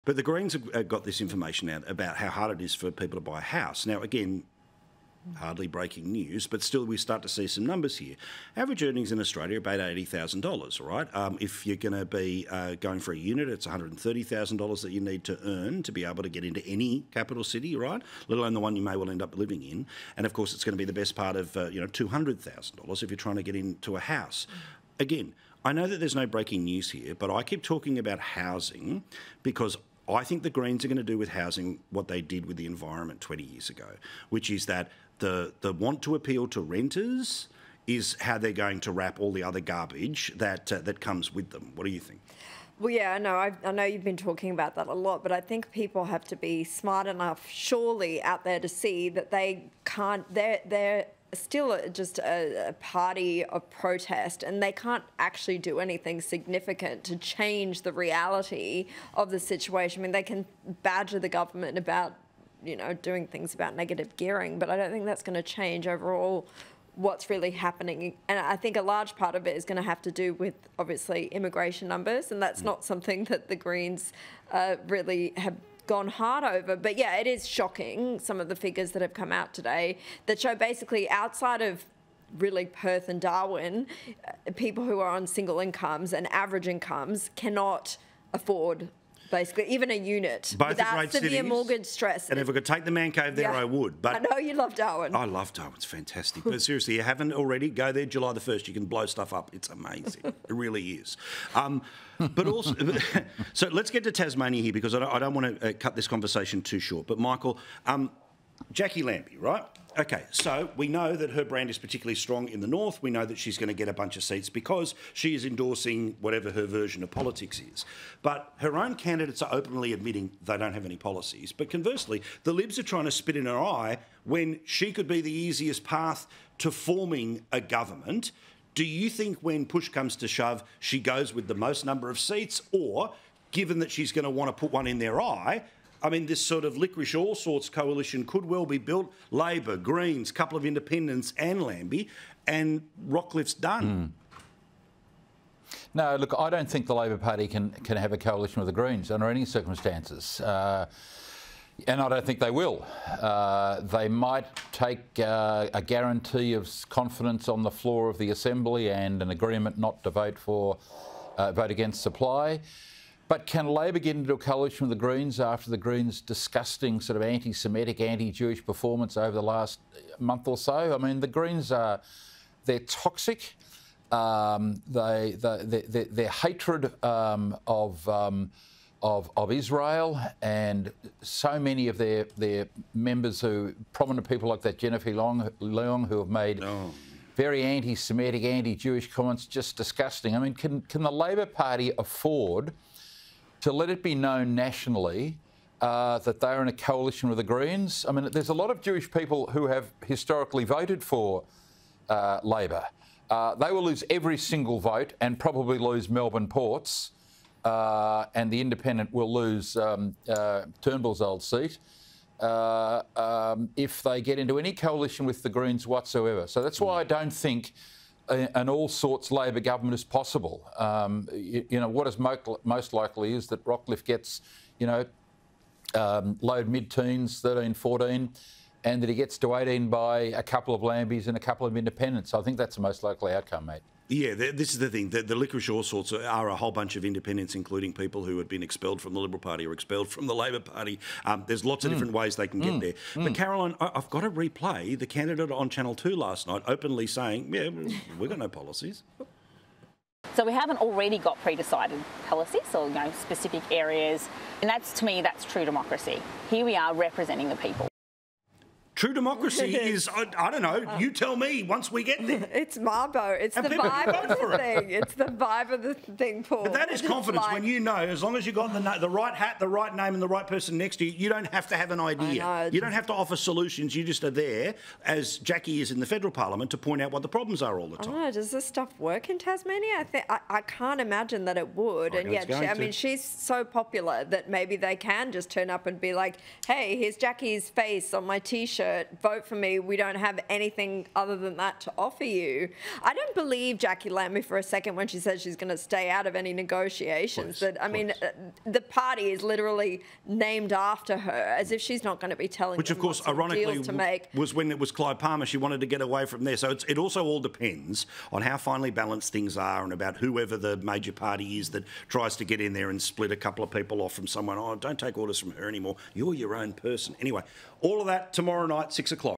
Recent data shows average Australian earners priced out of housing
A new analysis released by the Greens has revealed that affordable housing is beyond reach for the average Australian.

The report shows that the average annual income required to buy a home without financial stress is $164,400.

With figures dropping to $130,599 annual income needed to buy an apartment in Australia’s capital cities without severe mortgage stress.

“It is shocking some of the figures that have come out today,” Sky News Senior Reporter Caroline Marcus said.

“It shows basically outside of really Perth and Darwin people who are on a single income and average incomes cannot afford basically even a unit without severe mortgage stress,” she said.